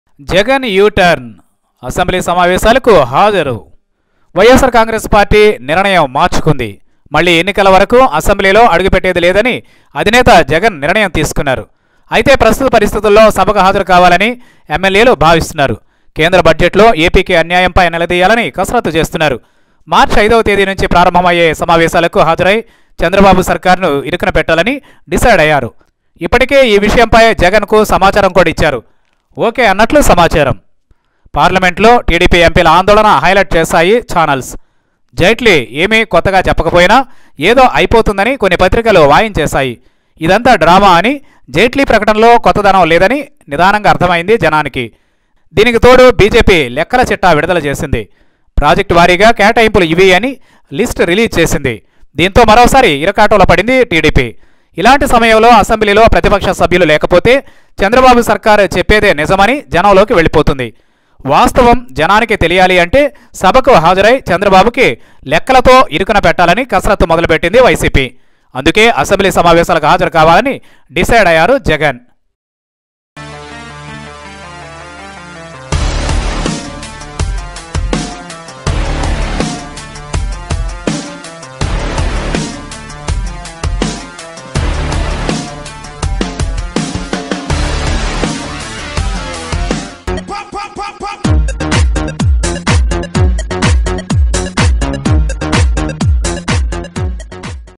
అసెంబ్లీ బహిష్కరణ నిర్ణయం మార్చుకున్న జగన్..!| YS Jagan Decided to Attend Next Assembly Sessions
YS Jagan Decided to Attend Next Assembly Sessions..!
YSR Congress Chief YS Jagan has Changed his Decision on Assembly expulsion. Now He Deceded to Attend Next Assembly Sessions, Which Start from March 5th.
Watch this video to know about the reason/
Amaravathi Media